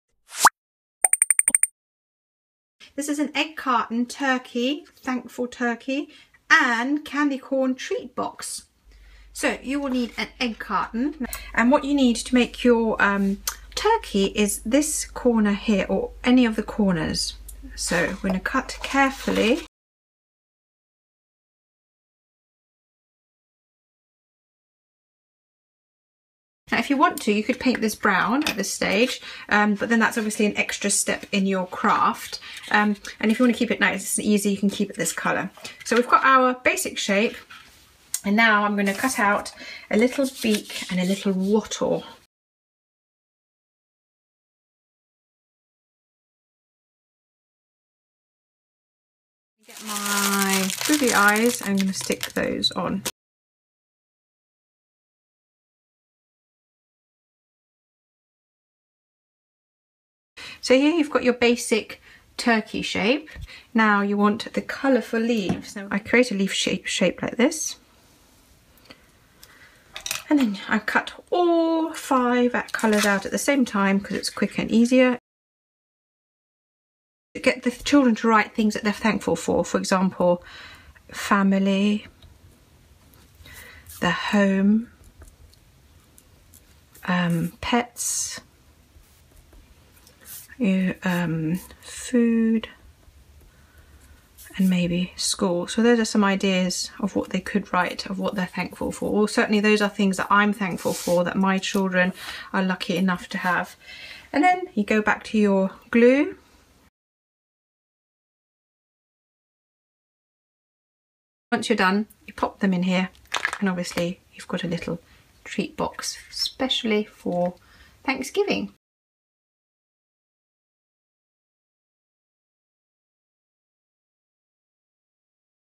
Egg Carton Turkey (Short Version) - how to make a thankful turkey craft
A simple Thanksgiving Craft for Kids - make this "Thankful Turkey" treat box. Oh I love this on so many levels - we are upcycling egg carton boxes to make this cute Turkey Craft. The kids get to think about and write what they are Thankful for, they make cute table decorations AND you can fill these turkeys with candy corn treats!!

This is a collaboration with the lovely and wonderful The Purple Alphabet. Check out her turkey activity as well as other turkey crafts in the collaboration!